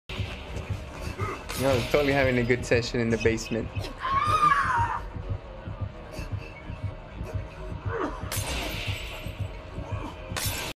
The Clown's Smoking Session With Claudette
It's almost that time of the year and the clown and Claudette know what to do. Happy 420!